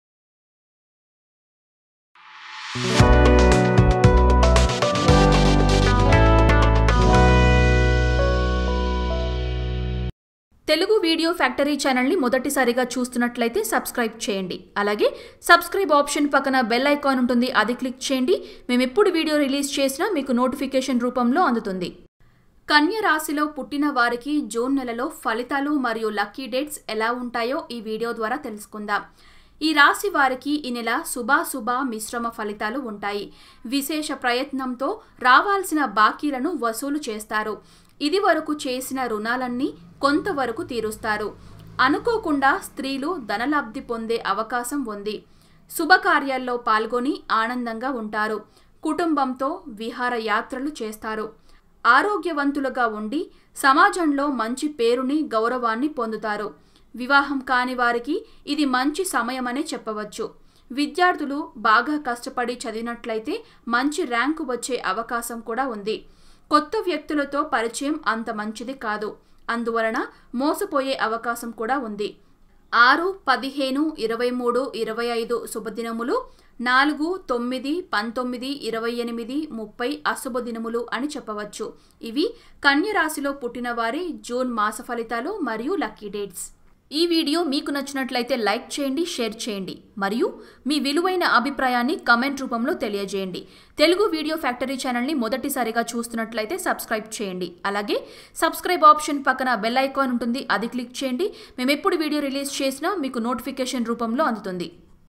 Kanya rasi june 2019 telugu|Rasi phalalu telugu|రాశి ఫలాలు|కన్యరాశి జూన్ నెల ఫలితాలు|Virgo Horoscope
Kanya rasi june 2019 telugu.
కన్యరాశి జూన్ నెల ఫలితాలు.
June month 2019 kanya Rasi phalalu in Telugu astrology.
In this video, we have explained about 2019 June month Rasi phalalu and predictions and lucky dates for kanya rasi people in the Telugu language according to astrology.